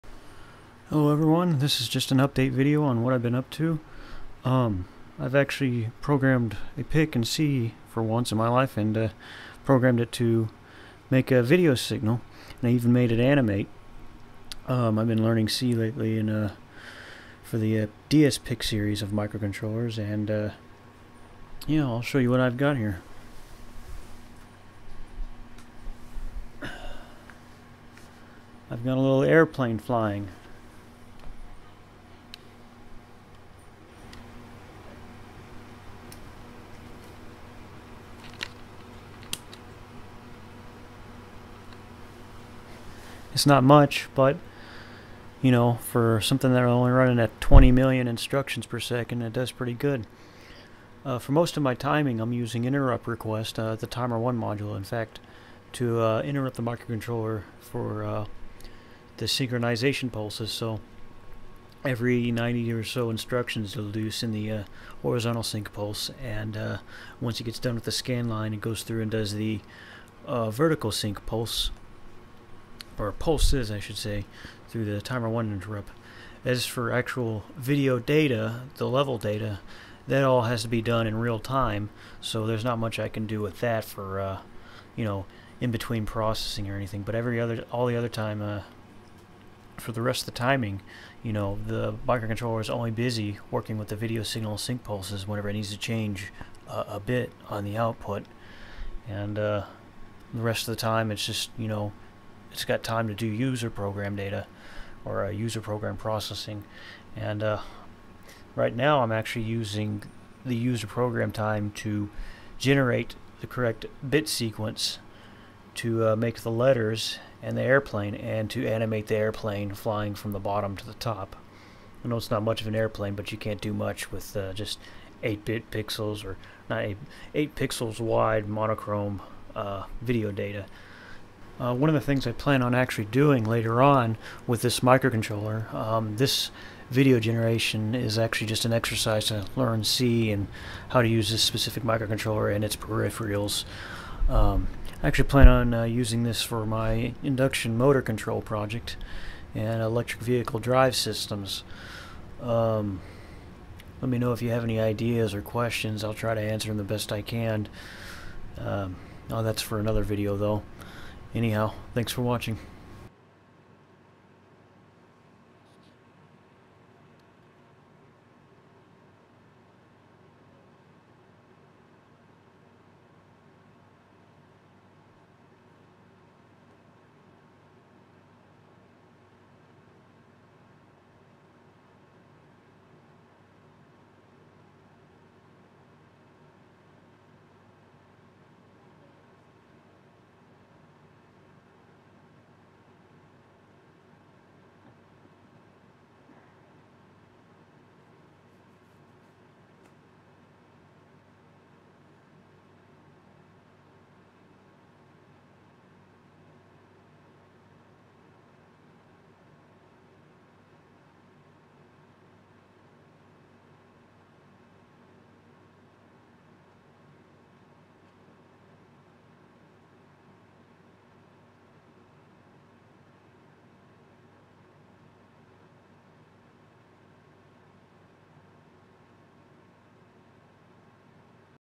dsPIC composite video output.
Video signal generation using a dsPIC30F3011 using the T1 timer and its IRQ. Running at 80MHZ for 20MIPS.

update July 2023: I found the old code I wrote to do this. It's awful. lol